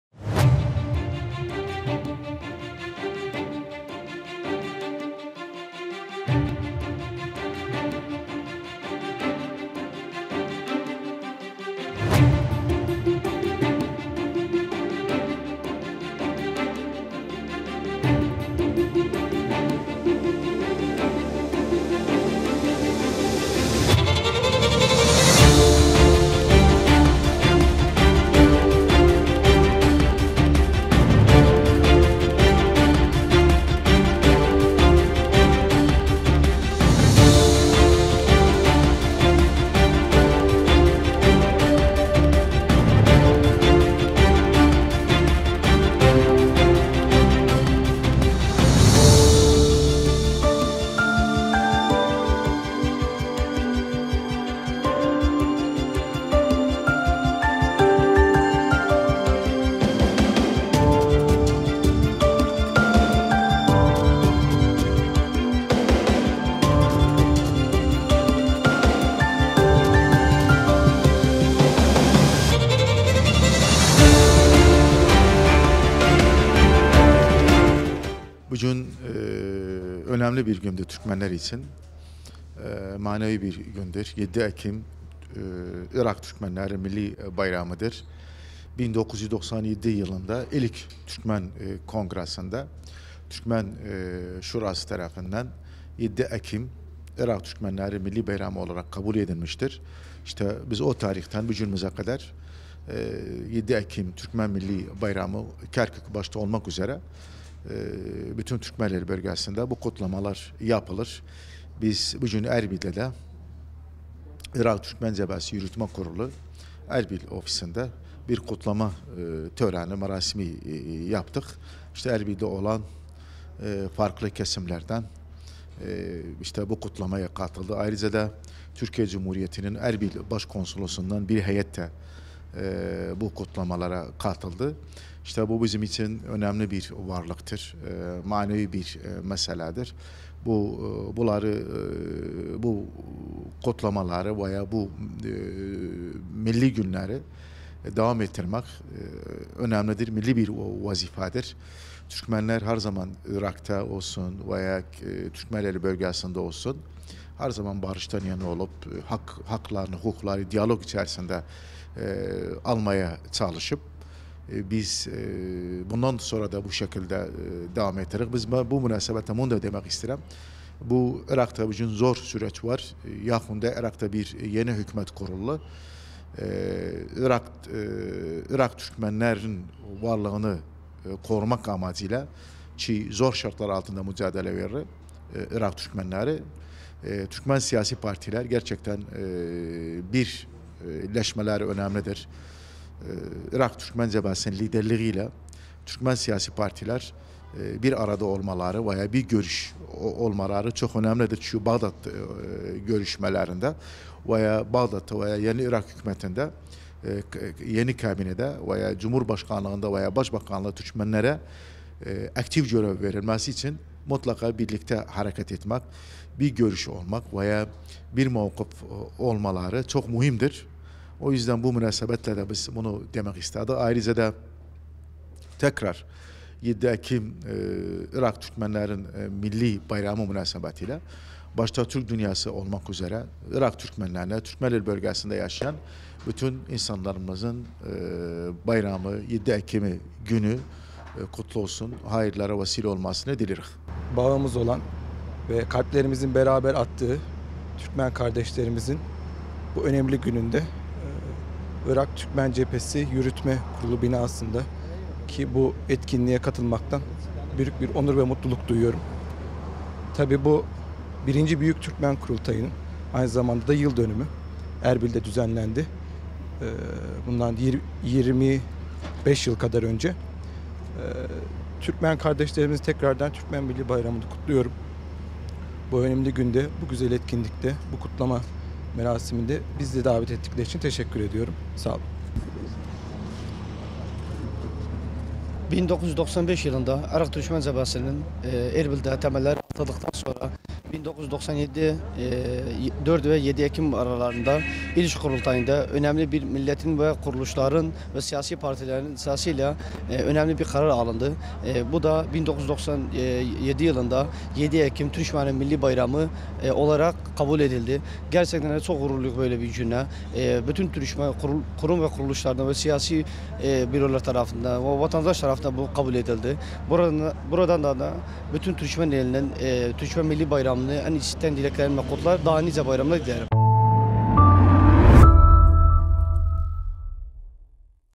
Türkmen Milli Bayramı Erbil'de kutlandı
Irak Türkmen Cephesi (ITC) Siyasi Bürosunda  7 Ekim Türkmen Milli Bayramı münasebetiyle kutlama gerçekleştirildi.

Törene, ITC Siyasi Büro Üyesi ve Irak Kürdistan Bölgesi Yönetimi (IKBY) Oluşumlardan Sorumlu Bölge Bakanı Aydın Maruf, ITC Siyasi Büro Üyesi ve ITC İl Başkanı Emir İzzet, ITC Milletvekili İmdat Bilal, ITC Erbil İl Başkanı Yardımcısı Fevzi Neccar, ITC Erbil kadrosu, aktivist ve halk katılım gösterdi.

7 Ekim Türkmen Milli Bayramı

7 Ekim Türkmen Milli Bayramı, Erbil’de 1997 yılında yapılan ilk Türkmen Kurultayı’nda ortak kararla milli bayram olarak kabul edildi.

1995 yılında kurulan Irak Türkmen Cephesi, 1997 yılında ilk Türkmen Kurultayı’nı Erbil'de gerçekleştirdi. Kurultay 4-7 Ekim tarihleri arasında yapılırken, burada Türkmen milleti adına önemli kararlar alındı.

Aynı zamanda söz konusu kararların alındığı 7 Ekim tarihi ise aynı günde, Türkmen Milli Bayramı olarak oylamaya sunuldu ve oy birliği ile kabul edildi.

Takipte Kalın...